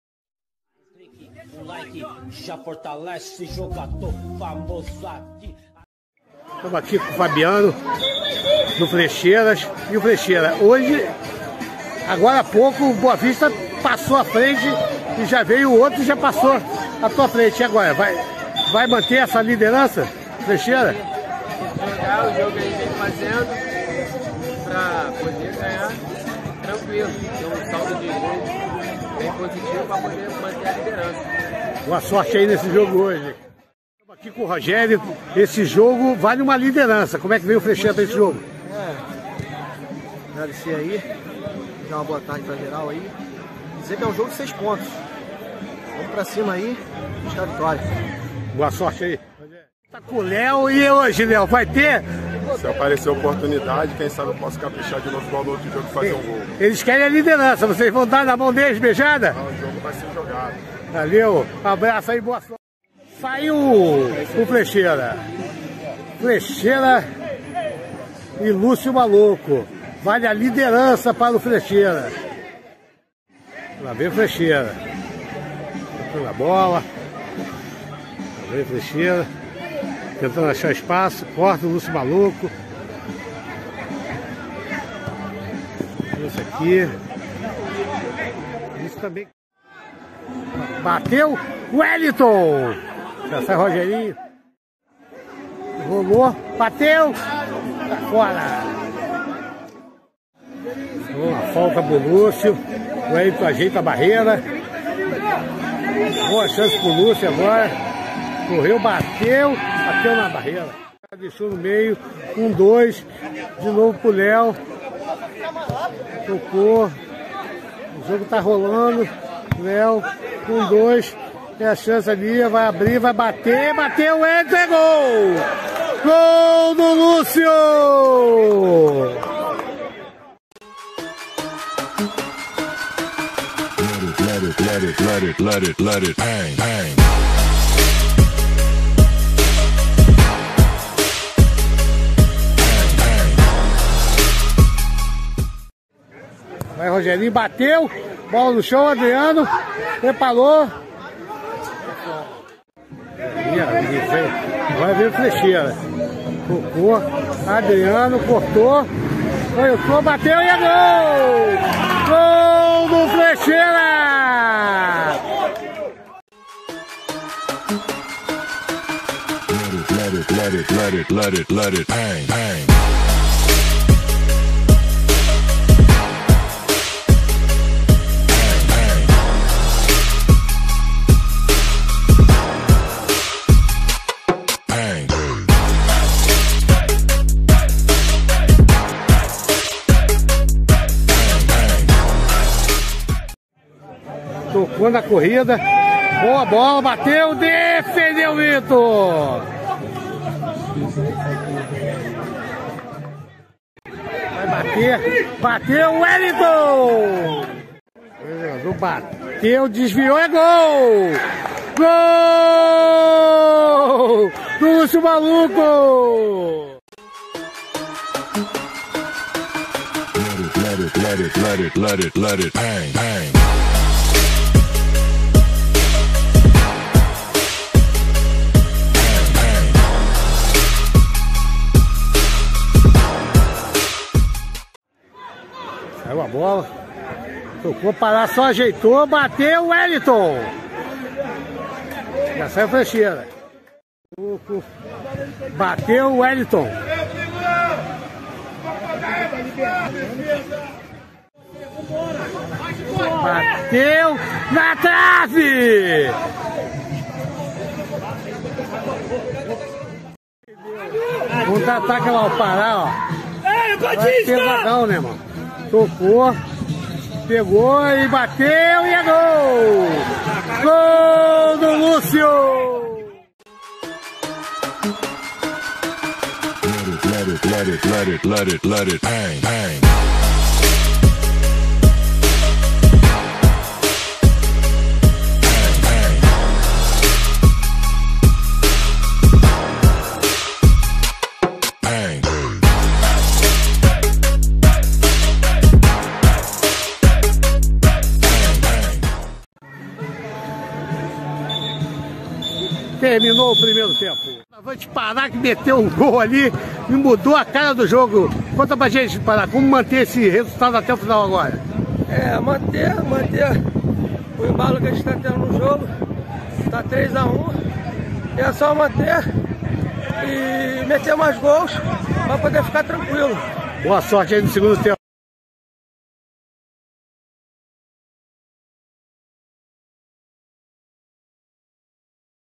LUCIO MALUCO X FLEXEIRAS Valendo Liderança LUCIO MALUCO avisa eu Volteeeei !!!
Jogador famoso.10
Jogador famoso na Penha RJ
Campeonato de Veteranos da Cintra
# LUCIO MALUCO X FLEXEIRAS
# Dupla dos Flexeiras sem sincronia
Lucio Maluco domina com toque de bola
#1° tempo
Gilvandro Silva
Andre Paes
26/03/2022